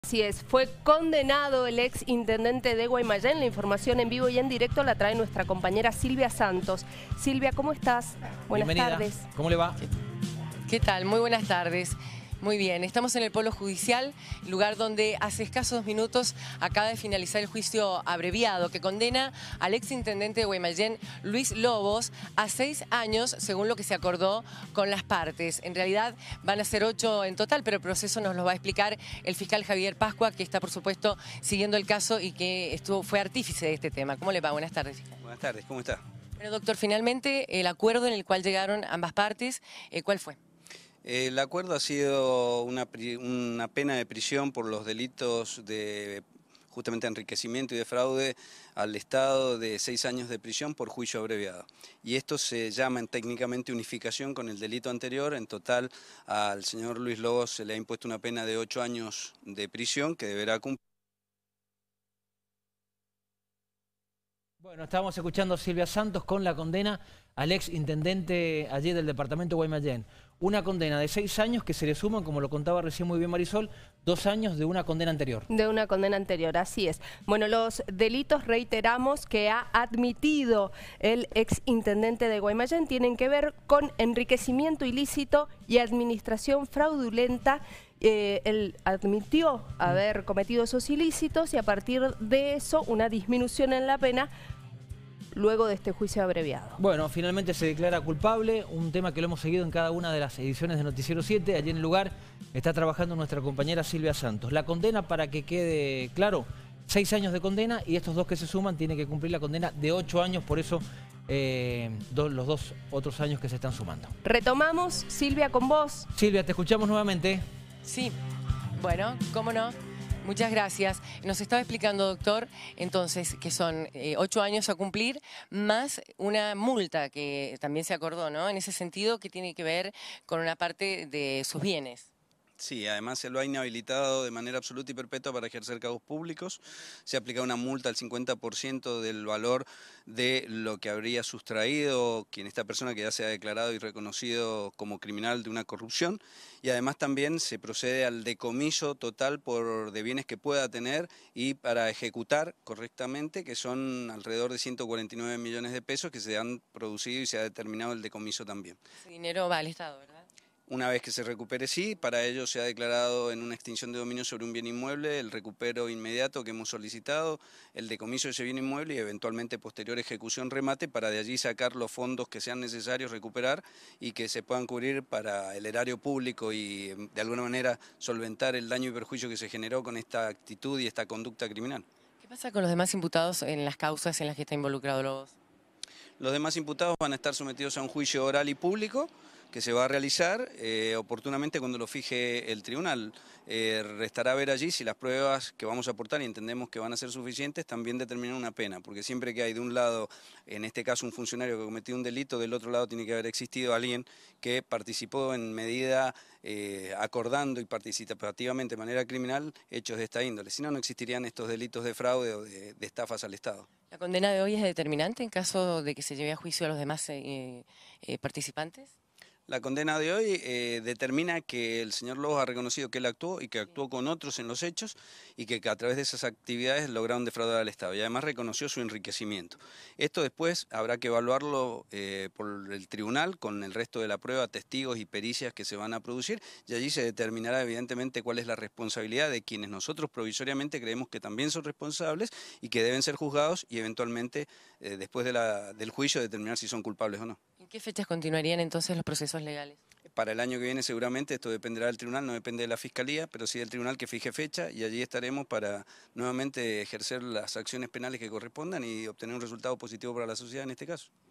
LUIS LOBOS FUE CONDENADO POR CORRUPCIÓN Y PASARÁ 8 AÑOS EN LA CÁRCEL
El ex intendente de Guaymallén reconoció ser autor de los delitos de corrupción